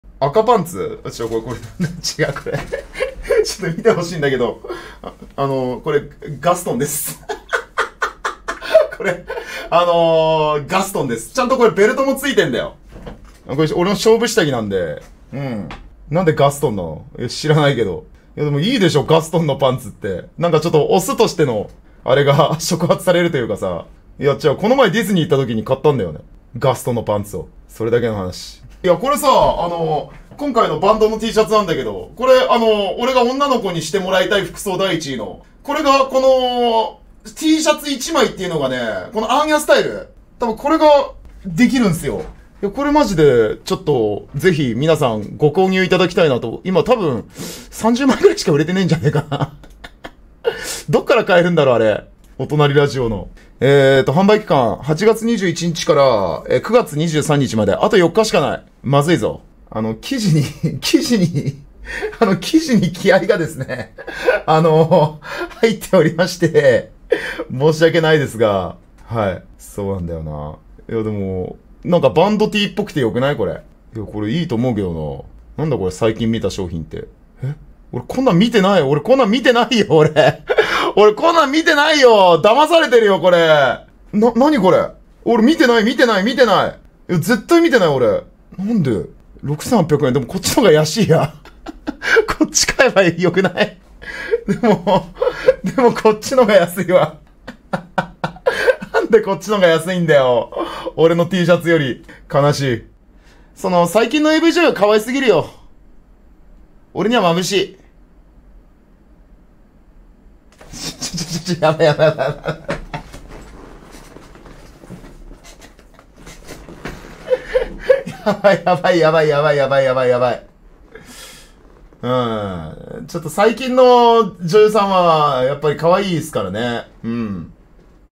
おとラジの「ヴィンテージ風バンドTシャツ」が引くほど売れてない件【2024/09/19】
元配信
ゆゆうた押忍